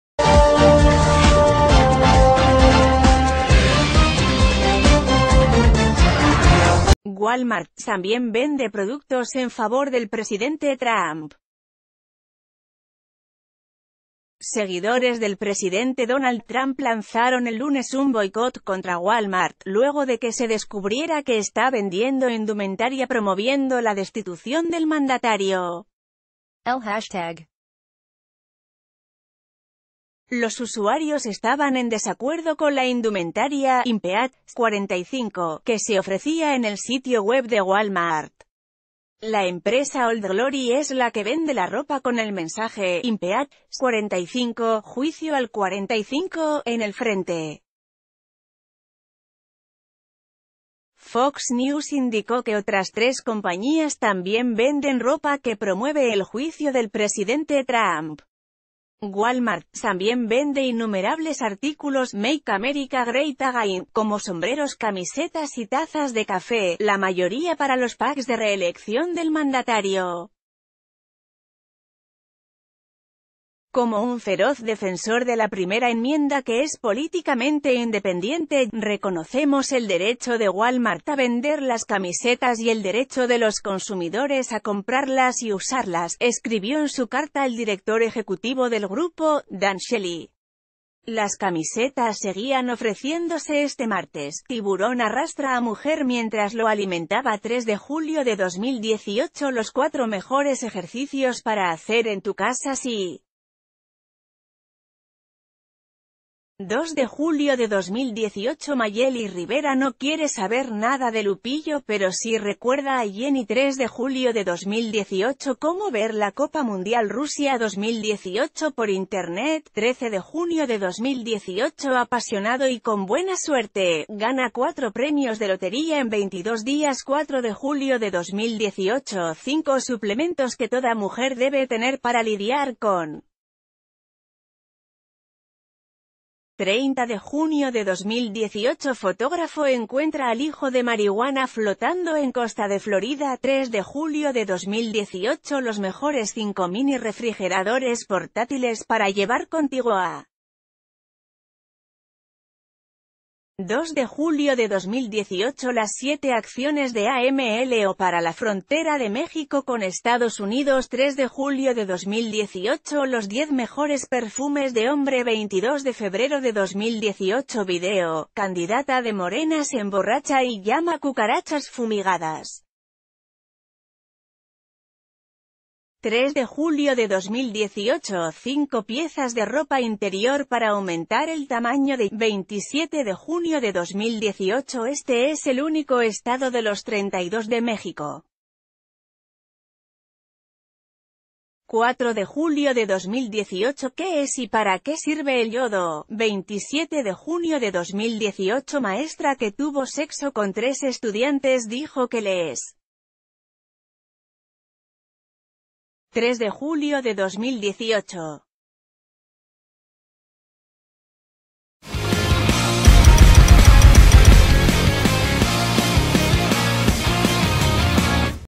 Arman boicot contra Walmart por vender camisetas anti Trump
Walmart también vende productos en favor del presidente Trump. Seguidores del presidente Donald Trump lanzaron el lunes un boicot contra Walmart, luego de que se descubriera que está vendiendo indumentaria promoviendo la destitución del mandatario. El hashtag BoycottWalmart se convirtió en una tend...